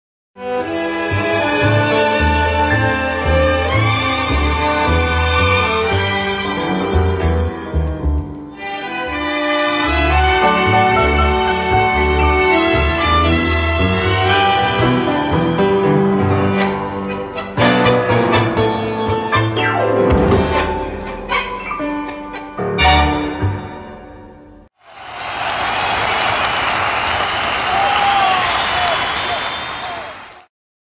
Tango - Dance Demonstration from Arthur Murray's in Sherman Oaks
"Staff from arthurmurraythebest.com in Sherman Oaks, California (91403) demonstrate a sampling of "Tango". See other clips in the series by going to Youtube user name: ArthurMurray91403".